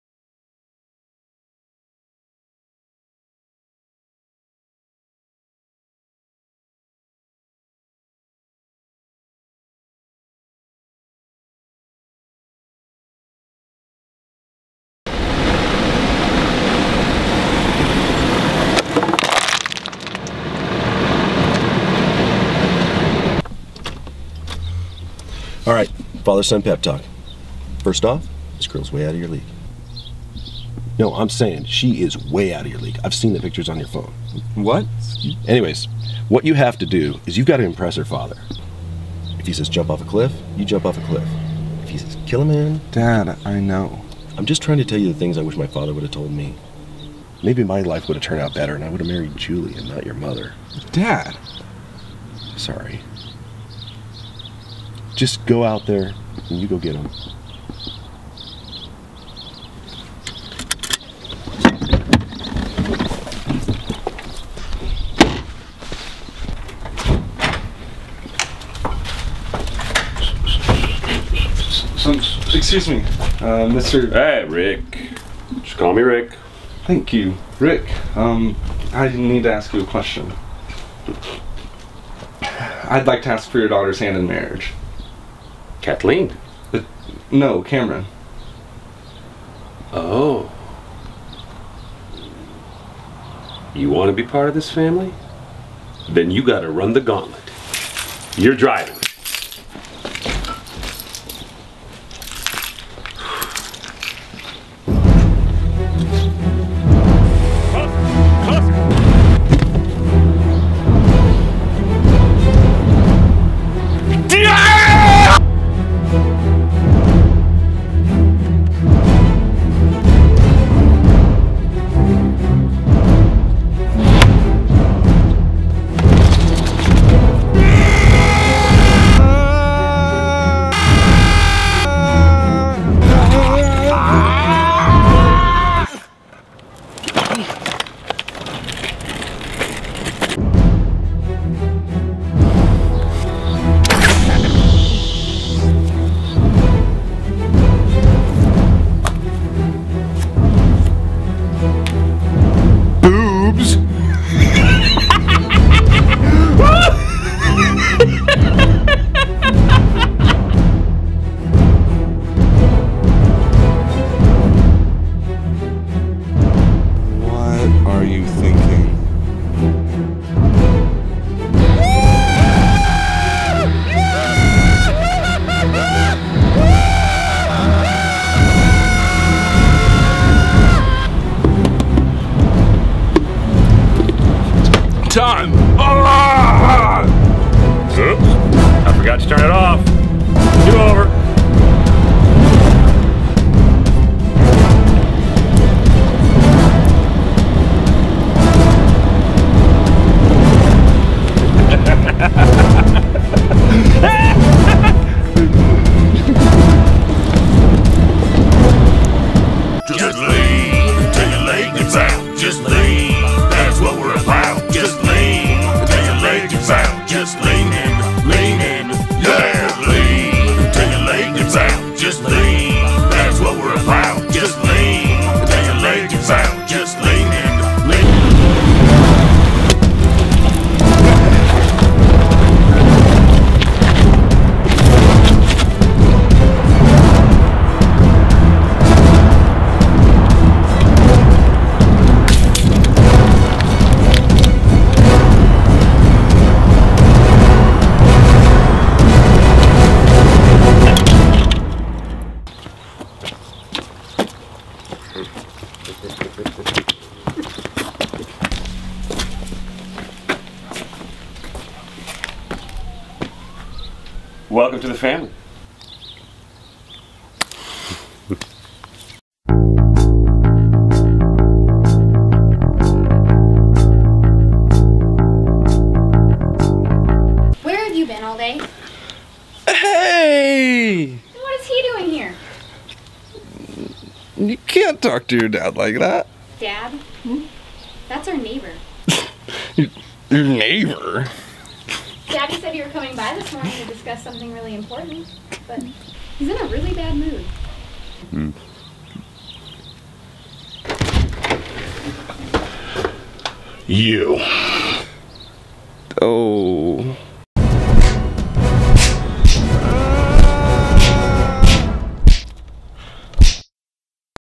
'The Gauntlet' (48 Hour Film)
In this film created for the August "North By Northwest" 48 Hour Film Contest, Doug wants to gain the approval of his potential fiancee's father. To do this, he must survive "The Gauntlet."

Team Eagle Omelet Is:
Dave Kotlan
Seth Mead
Keegan McClung
Doug Ault
Milton Harper